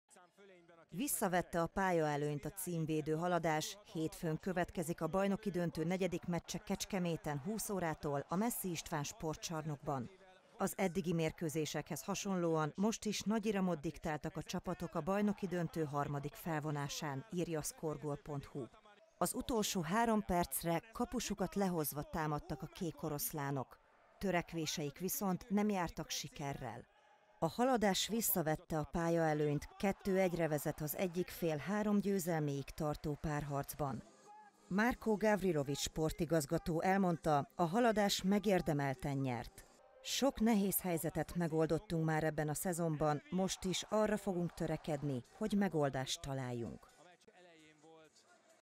Harcias meccsen fordított a címvédő
Visszavette a pályaelőnyt a címvédő Haladás, hétfőn következik a bajnoki döntő negyedik meccse Kecskeméten 20 órától a Messzi István Sportcsarnokban.

6000 Kecskemét, Szabadság tér 2.